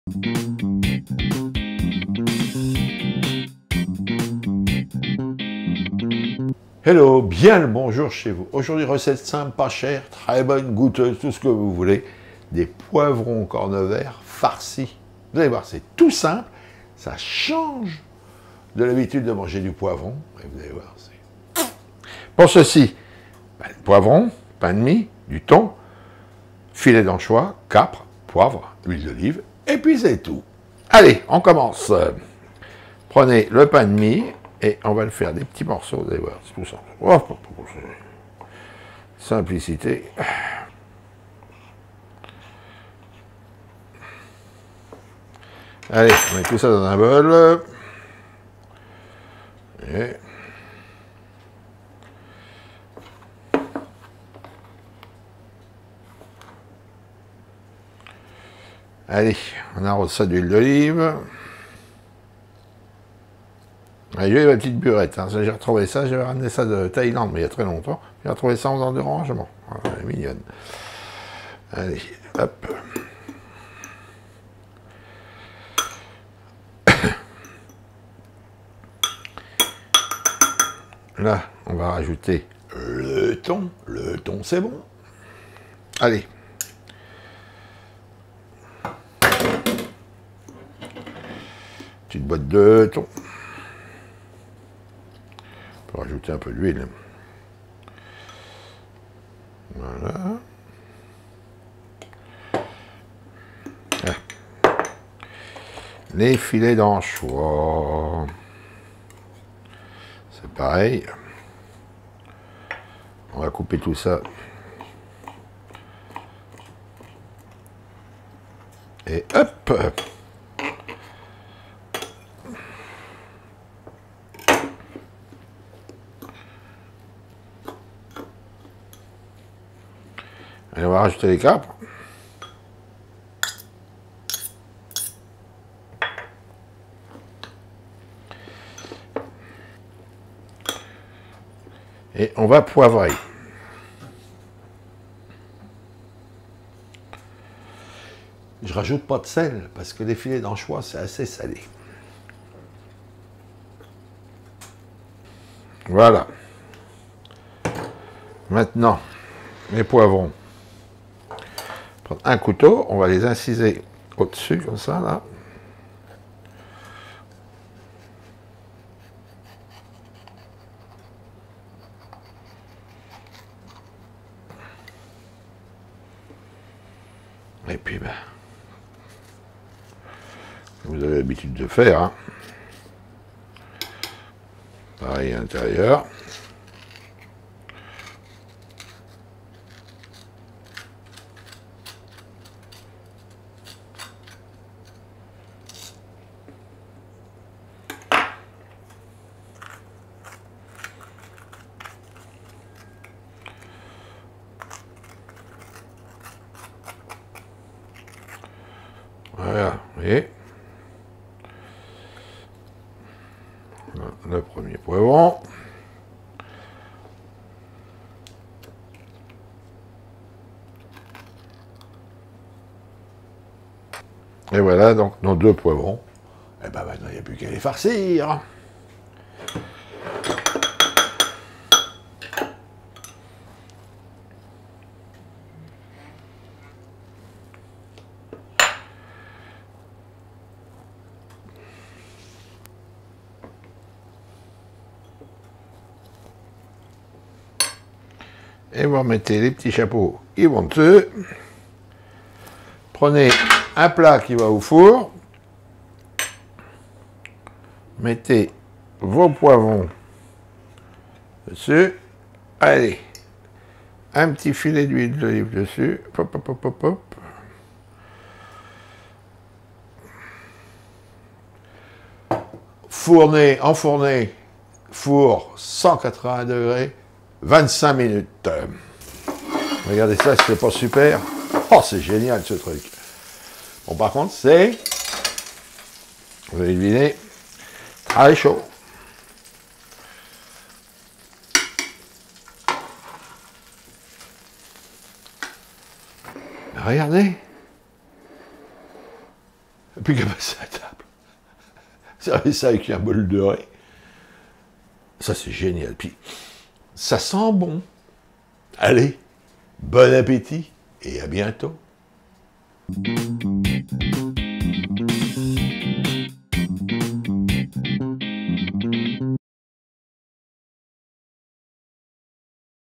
Poivrons corne vert farcis - Couverts et Baguettes
2 poivrons corne vert
2 tranches de pain de mie
1 Bt. de thon
2 cas de câpres
poivre
huile d'olive

PREPARATION :
Couper en petit dés le pain de mie
Dans un bol mettre les dés de pain de mie arroser d’huile d’olive, mélanger, ajouter le thon, mélanger, couper les filets d’anchois très finement puis les incorporer à la préparation, ajouter les câpres, poivré, bien mélanger.
Découper une ouverture sur chaque poivron, les vider puis les remplir avec la préparation, remettre  le chapeau, les disposer sur un plat allant au four les arroser d’huile d’olive et enfourner four chaud 180° 25 minutes.
Servir chaud